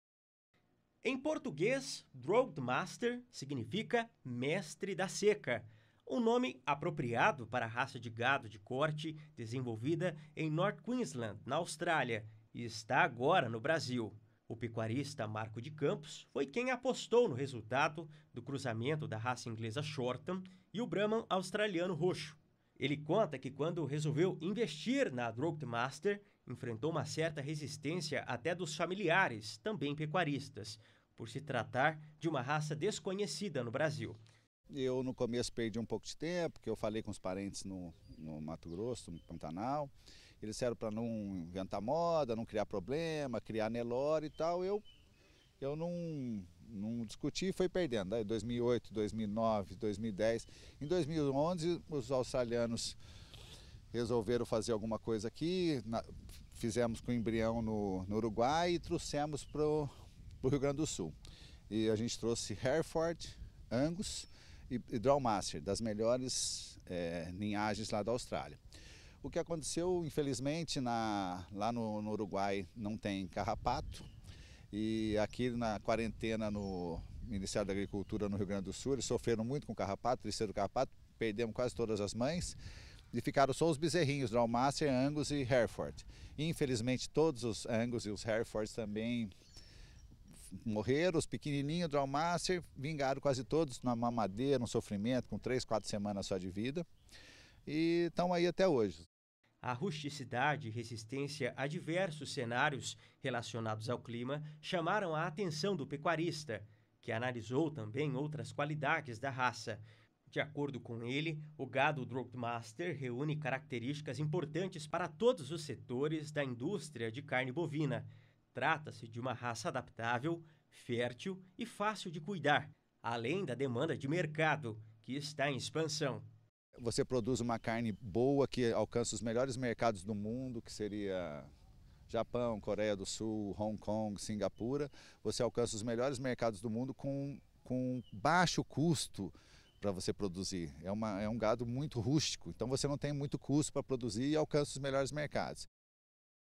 Droughtmaster chega ao Brasil apresentando gado mais rústico
Um pecuarista sul mato-grossense resolveu apostar em uma nova raça. Com origem australiana a Droughtmaster foi trazida ao Brasil e as qualidades desse gado, quando o assunto é resistência e rusticidade, chamam a atenção de qualquer pessoa. Conheça essa raça na reportagem de Cléber Rabelo.
Assista a nossos canais online, 100% grátis, clicando